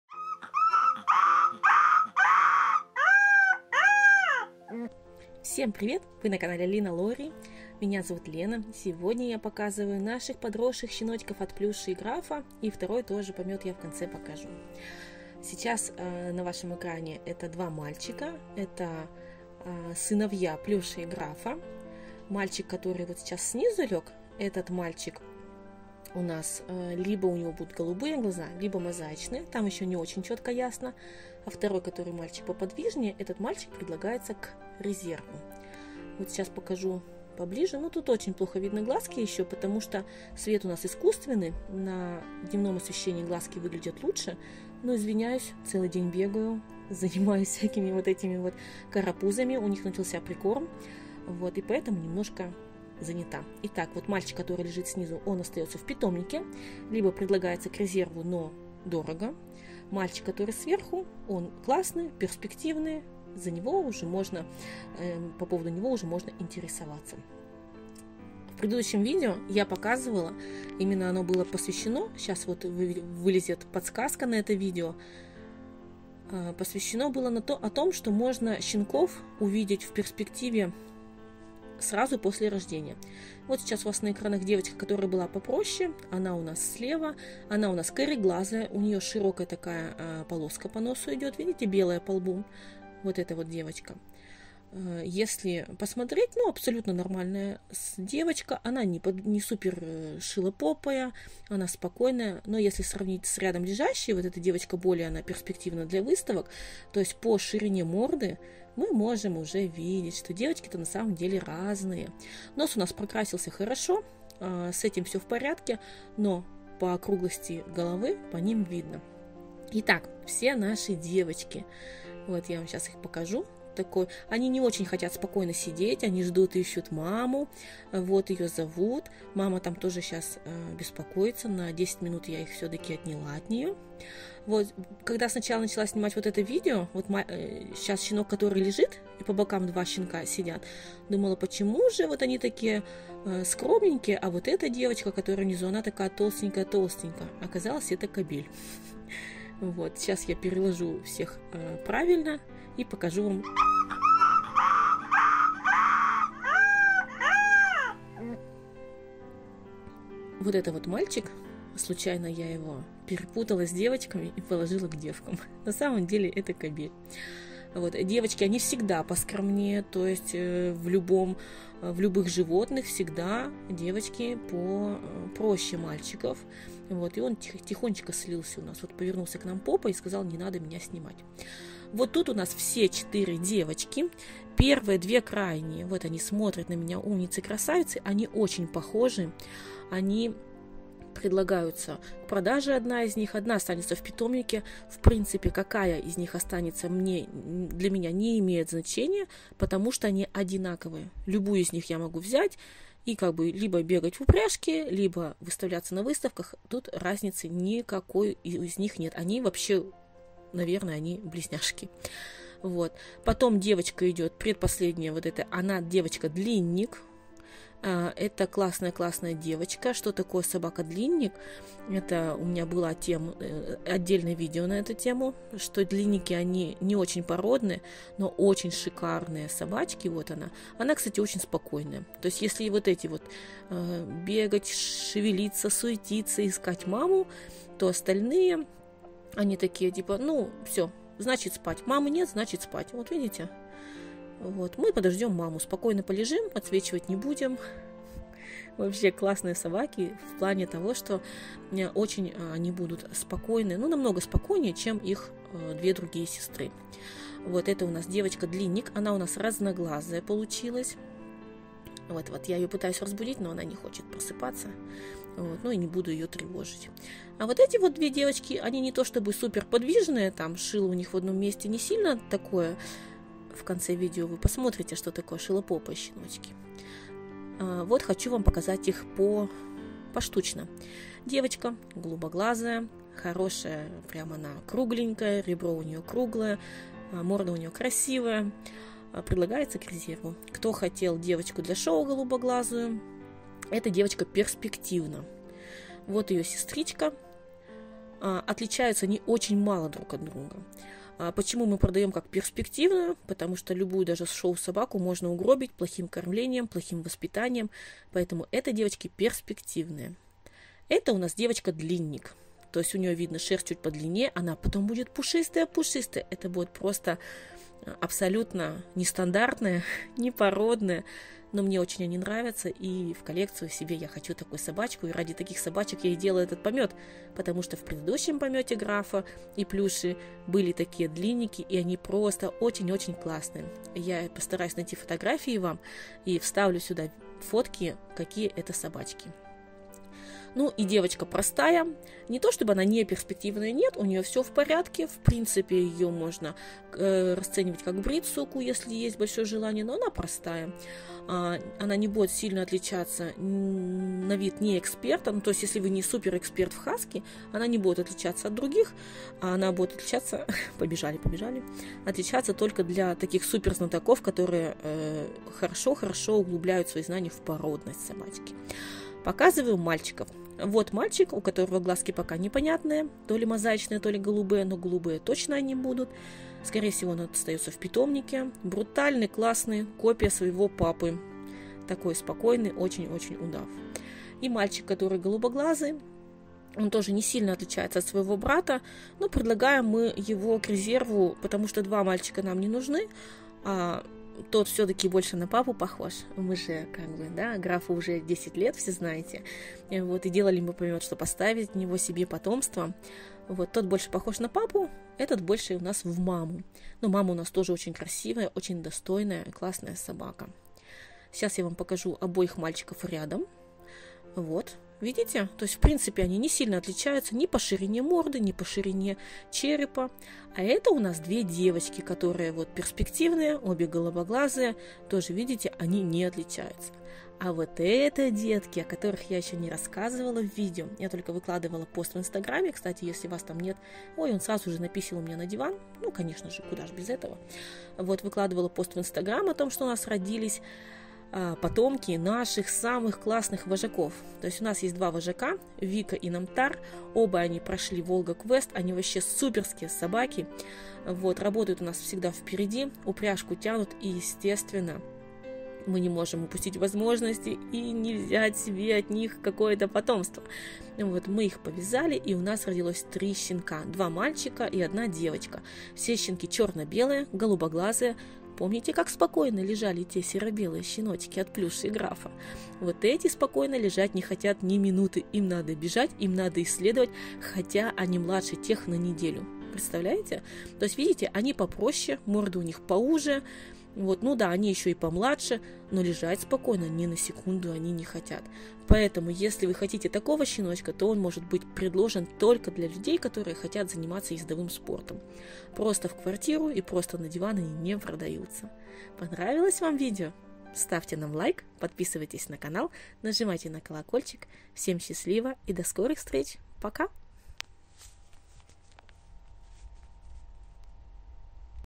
Щенкам 3 недели. Щенкам Вики 2 недели.
Ну вот и подросли хаски детки Плюши и Графа. Им 3 недельки.
Как они отличаются? Видны ли различия. Сравниваем и показываю какие предлагаются к продаже.
В первые показываю Ездовых хаски щенков. Отличаются от шоу?
все в видео
Пишите в комментариях кто больше понравился.